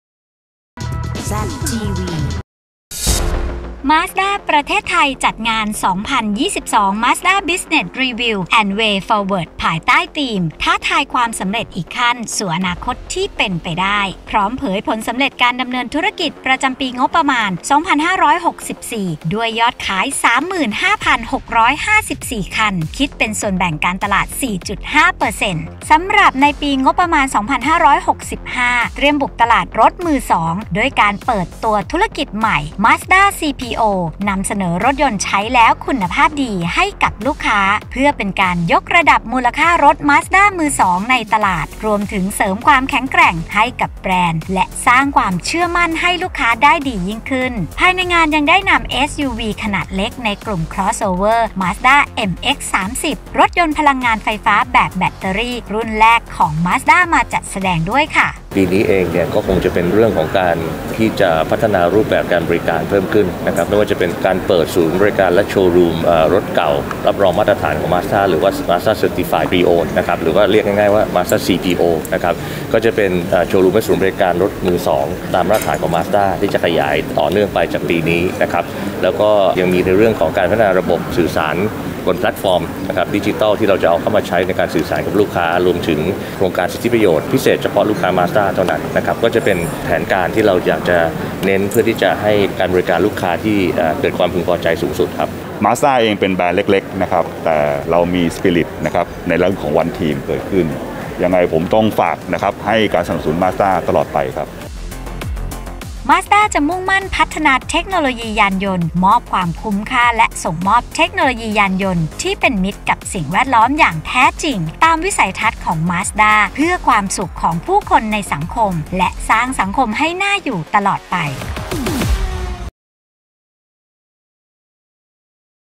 Z News “ 2022 Mazda Business Review & Way Forward ” เผยผลสำเร็จปี64 และ แผนงาน ปี65 10/4/65
มาสด้า ประเทศไทย จัดงาน “2022 Mazda Business Review & Way Forward” ภายใต้ธีม “ท้าทายความสำเร็จอีกขั้น…สู่อนาคตที่เป็นไปได้”พร้อมเผยผลสำเร็จการดำเนินธุรกิจประจำปี 2564 และแผนงานธุรกิจประจำปี 2565

Instargram : ztvthailand